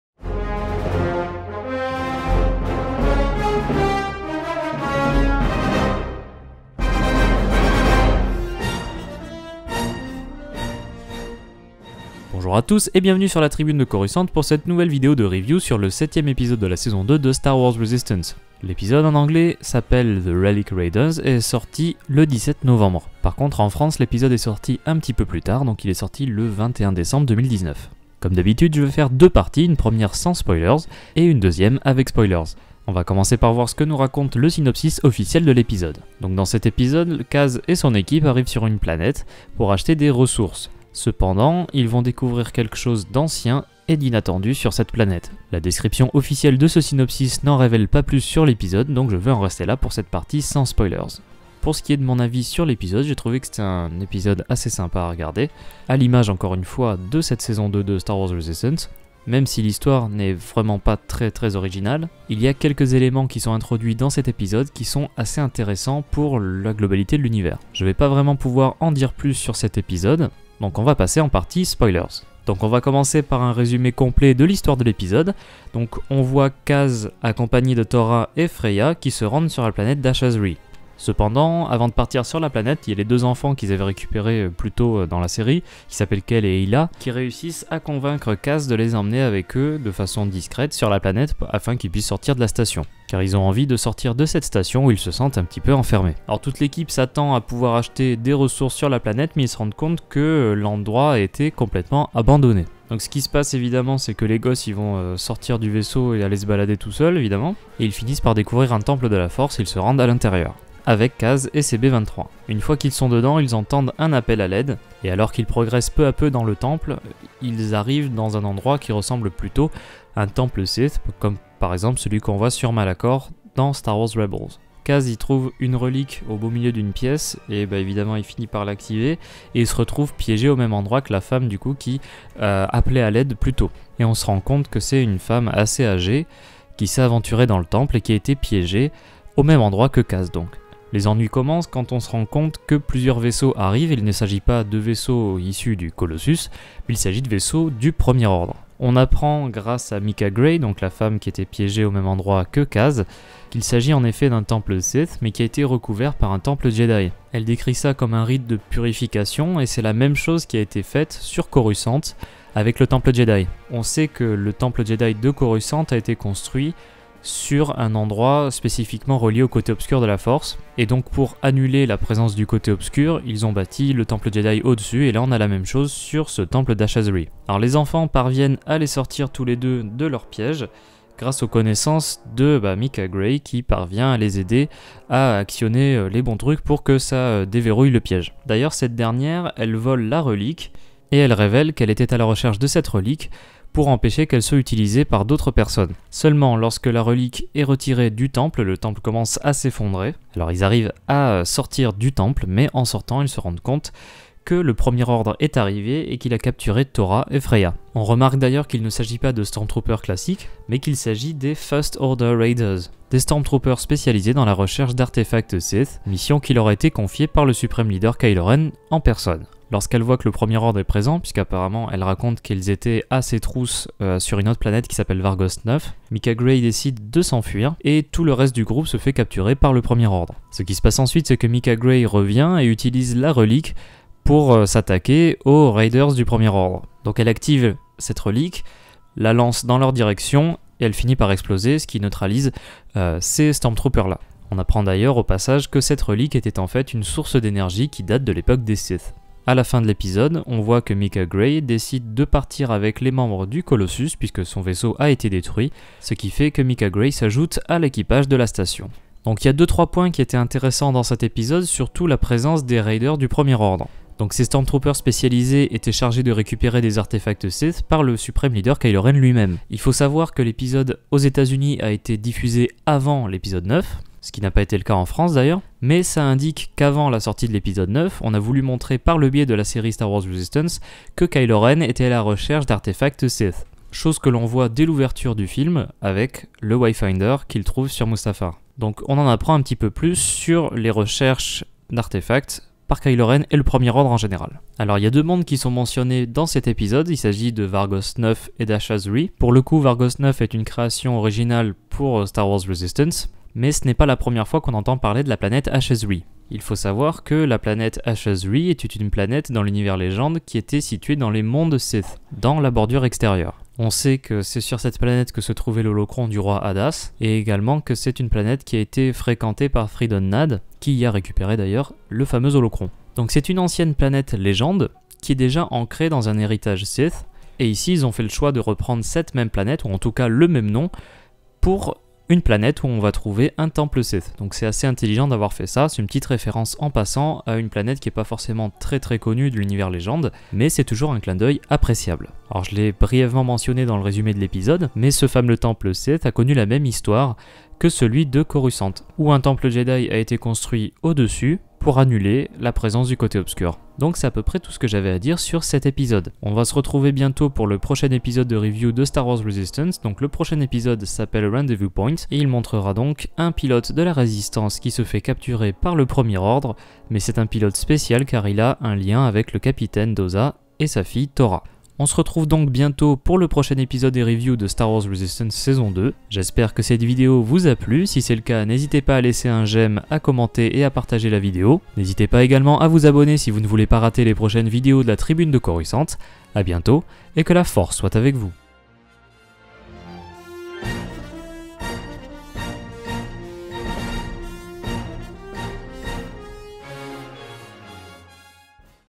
STAR WARS RESISTANCE S02E07 - The Relic Raiders (REVIEW) - La Tribune de Coruscant
Voici notre review du septième épisode de la Saison 2 de STAR WARS RESISTANCE. Attention évidemment aux spoilers !

Pour acquérir le Badge de la Tribune de Coruscant !